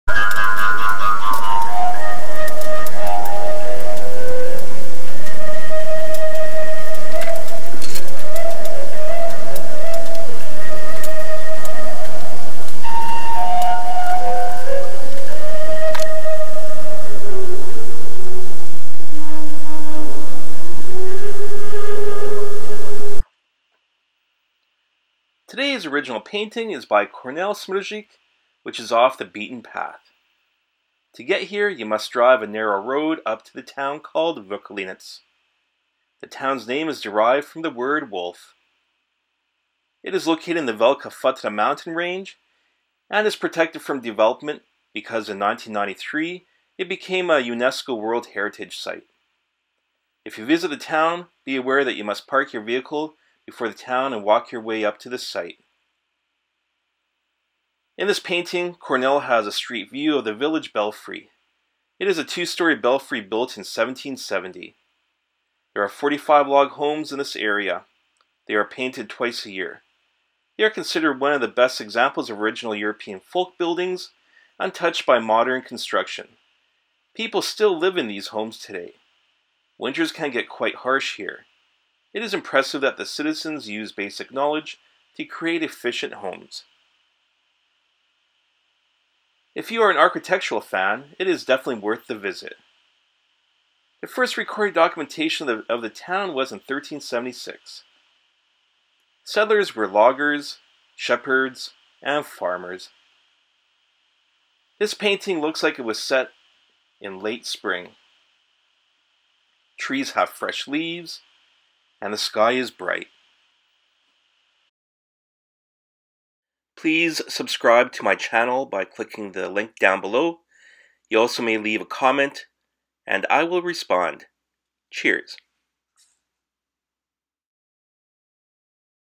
Vlkolinec - Original painting by Kornel Smrzik - Slovak Artist
Vlkolinec is a special UNESCO town in the Liptov region of Slovakia. I talk about the town in my review of his painting.